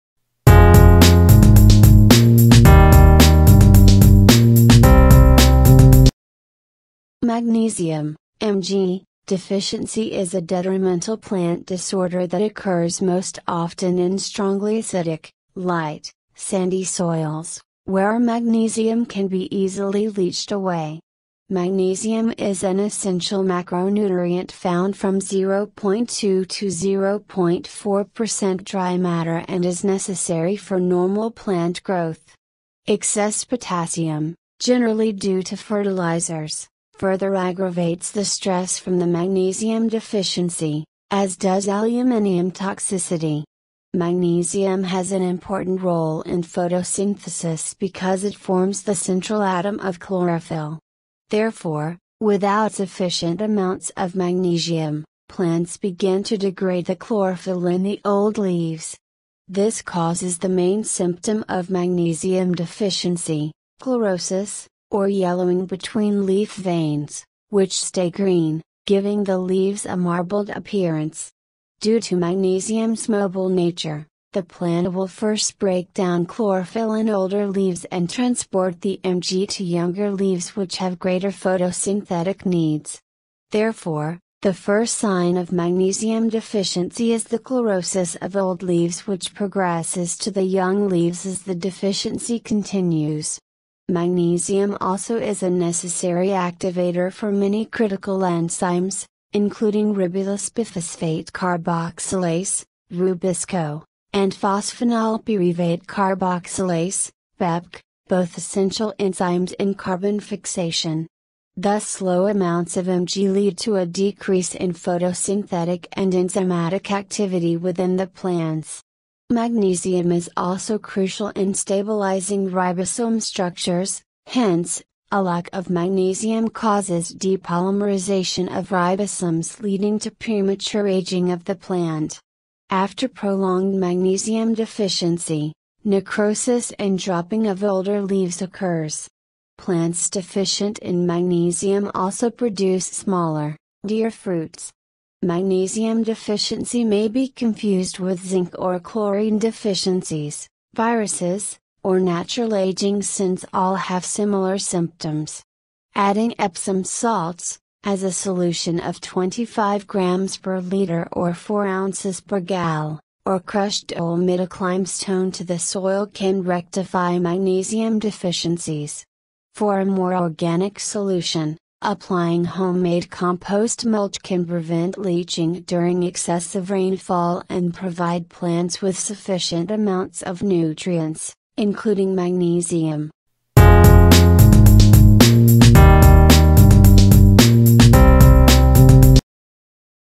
Magnesium deficiency plants
Magnesium (Mg) deficiency is a detrimental plant disorder that occurs most often in strongly acidic, light, sandy soils, where magnesium can be easily leached away. Magnesium is an essential macro nutrient found from 0.2-0.4% dry matter and is necessary for normal plant growth.
Excess potassium, generally due to fertilizers, further aggravates the stress from the magnesium deficiency, as does aluminium toxicity.

Magnesium has an important role in photosynthesis because it forms the central atom of chlorophyll. Therefore, without sufficient amounts of magnesium, plants begin to degrade the chlorophyll in the old leaves. This causes the main symptom of magnesium deficiency, chlorosis, or yellowing between leaf veins, which stay green, giving the leaves a marbled appearance. Due to magnesium’s mobile nature, the plant will first break down chlorophyll in older leaves and transport the Mg to younger leaves which have greater photosynthetic needs. Therefore, the first sign of magnesium deficiency is the chlorosis of old leaves which progresses to the young leaves as the deficiency continues. Magnesium also is a necessary activator for many critical enzymes, including ribulosbiphosphate carboxylase (RuBisCO) and phosphoenolpyruvate carboxylase (PEPC), both essential enzymes in carbon fixation. Thus low amounts of Mg lead to a decrease in photosynthetic and enzymatic activity within the plants. Magnesium is also crucial in stabilizing ribosome structures, hence, a lack of magnesium causes depolymerization of ribosomes leading to pre-mature aging of the plant. After prolonged magnesium deficiency, necrosis and dropping of older leaves occurs. Plants deficient in magnesium also produce smaller, woodier fruits.

Magnesium deficiency may be confused with zinc or chlorine deficiencies, viruses, or natural aging since all have similar symptoms. Adding Epsom salts (as a solution of 25 grams per liter or 4 oz per gal) or crushed dolomitic limestone to the soil can rectify magnesium deficiencies. For a more organic solution, applying home-made compost mulch can prevent leaching during excessive rainfall and provide plants with sufficient amounts of nutrients, including magnesium.